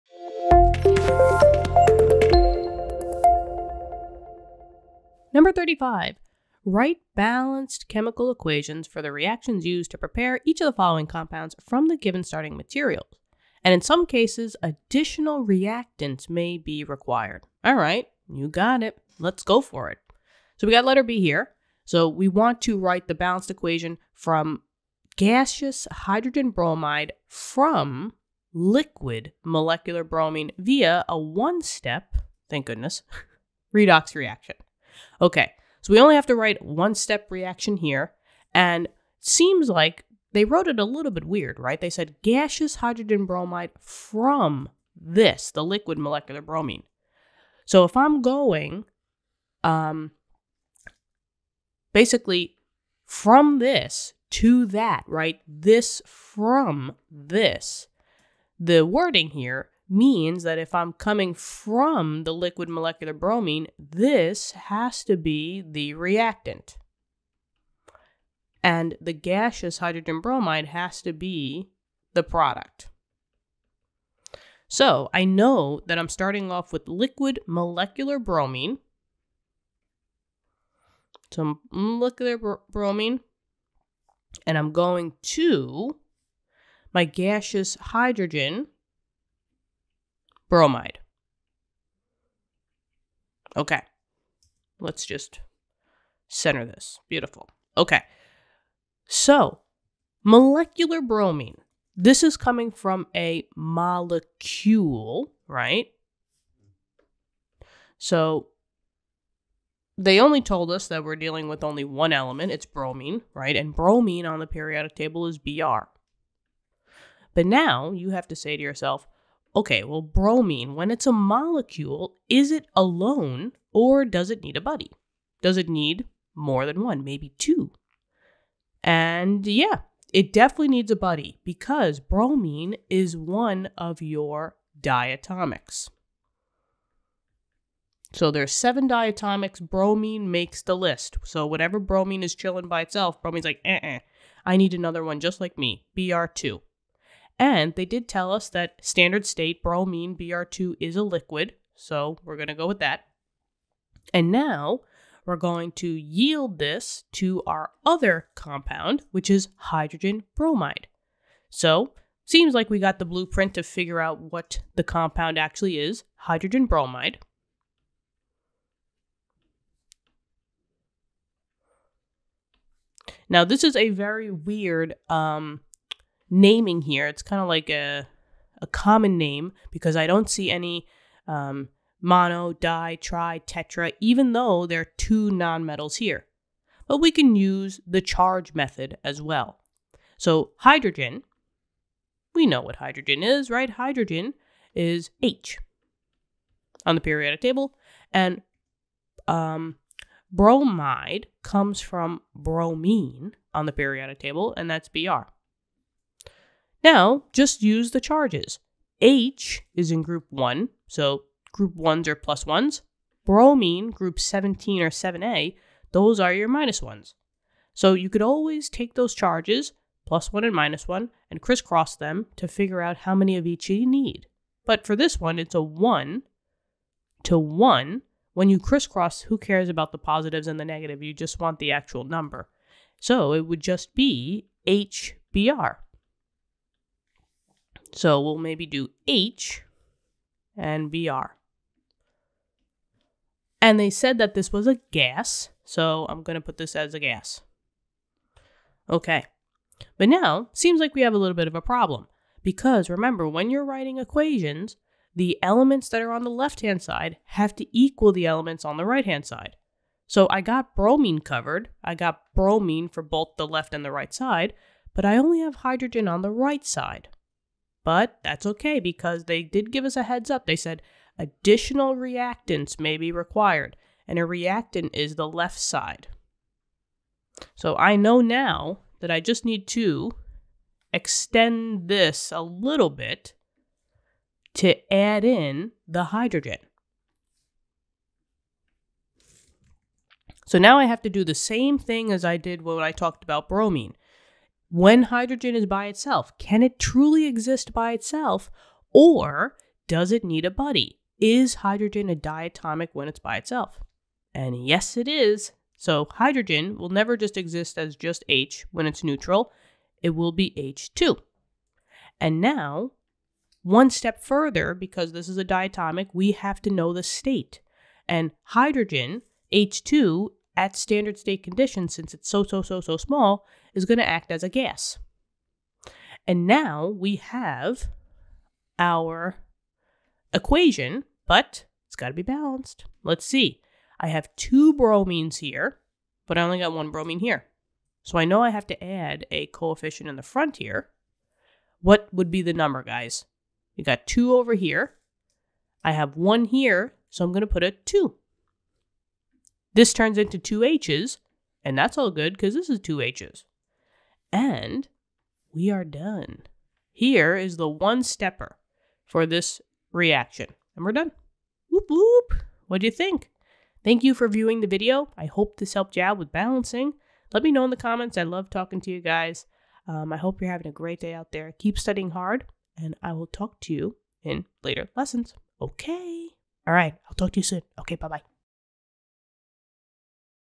4.35b | Gaseous hydrogen bromide from liquid molecular bromine via a one-step redox reaction
Write balanced chemical equations for the reactions used to prepare each of the following compounds from the given starting material(s). In some cases, additional reactants may be required.
(b) gaseous hydrogen bromide from liquid molecular bromine via a one-step redox reaction

BalancingEquations # BalancingChemistry#OpenStaxChemistry